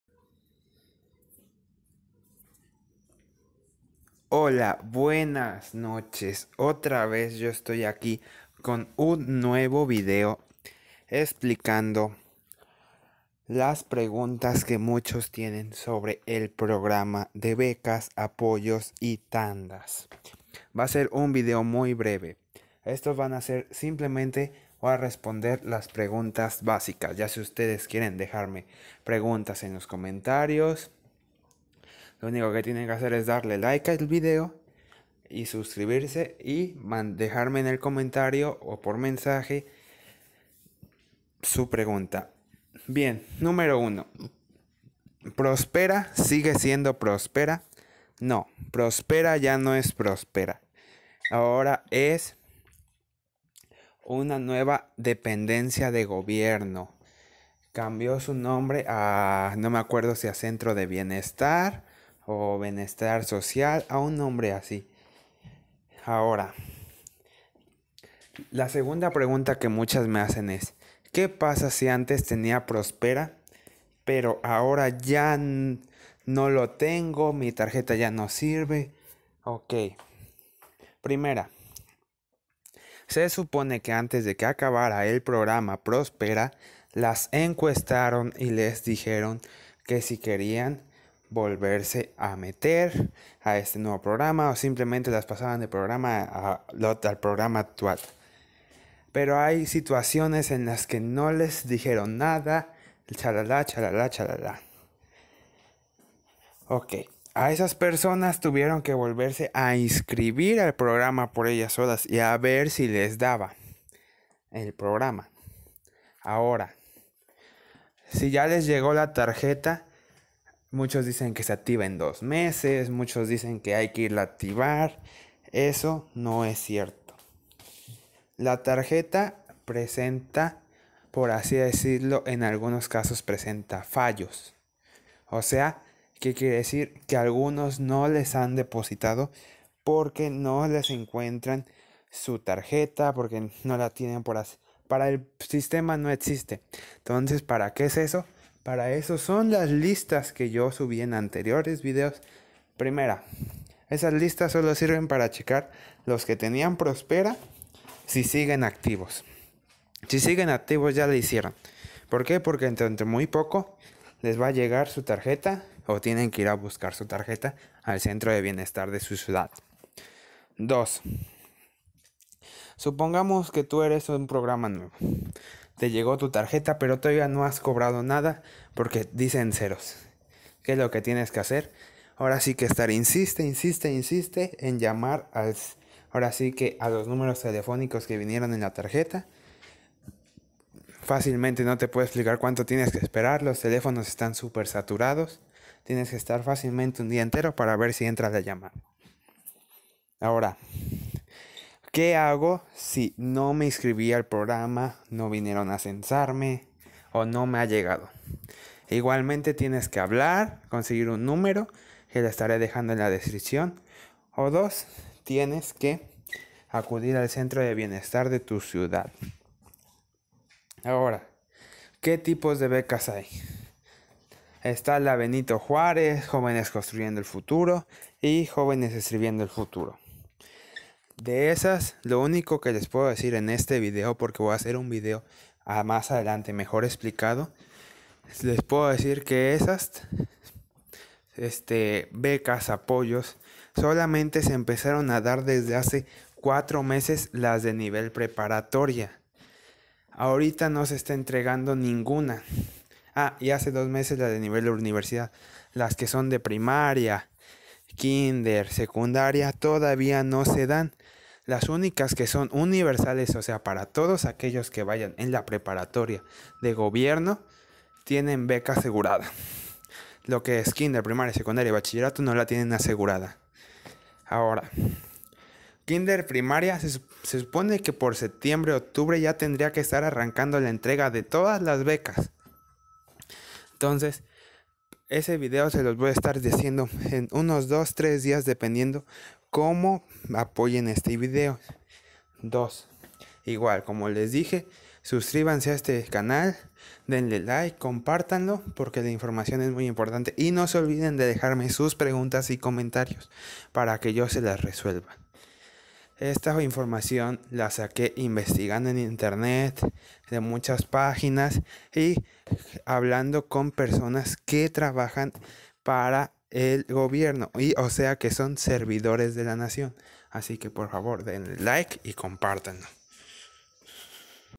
Respondiendo preguntas acerca del programa Prospera(CENTRO DE BIENESTAR) Tarjeta No Activada
Hola amigos aquí les dejo un nuevo video espero les guste, cualquier duda no duden dejarme la en los comentarios.
Nuevo video ¿Que es prospera?
¿Ahora centro de bienestar?
¿No me han depositado?
¿Mi tarjeta no ha llegado?
¿Cómo me inscribo?
Todas las dudas no olviden dejarme las en los comentarios para en el próximo video responderlas!